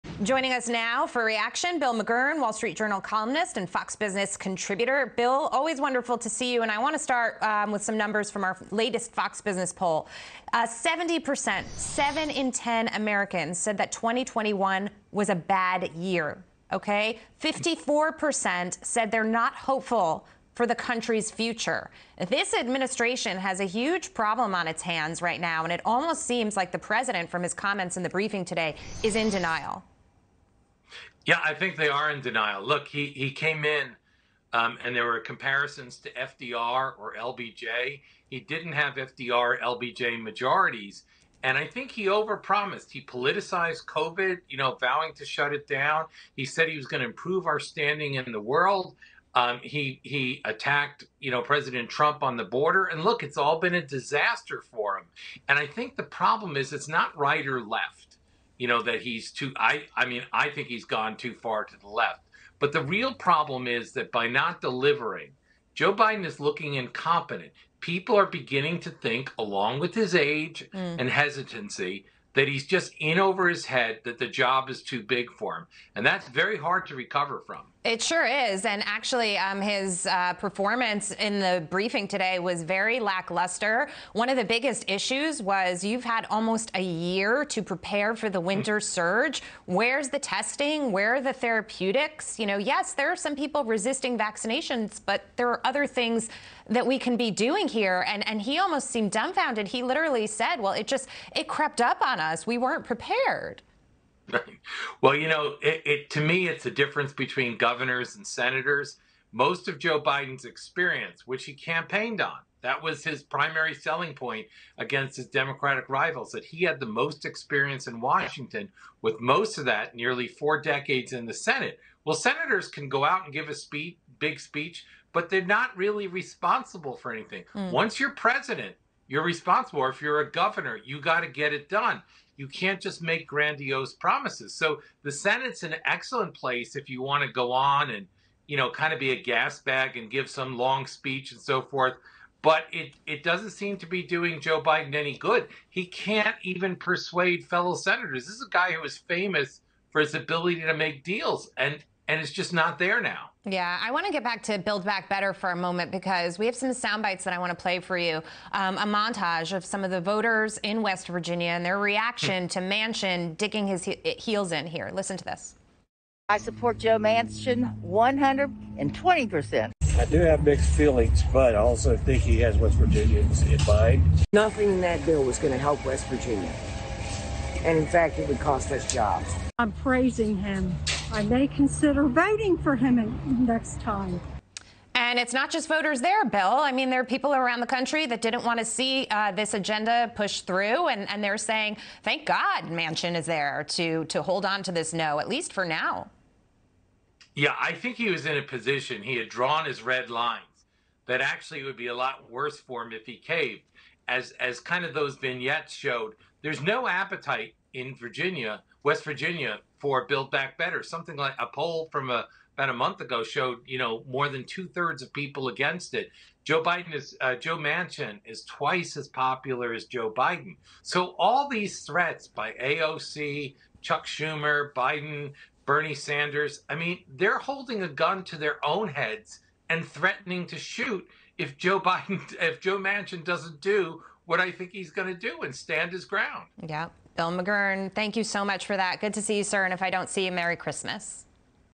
Democrats are in denial about this: McGurn
FOX Business contributor Bill McGurn discusses a recent poll showing a majority of Americans are not hopeful about the country’s future.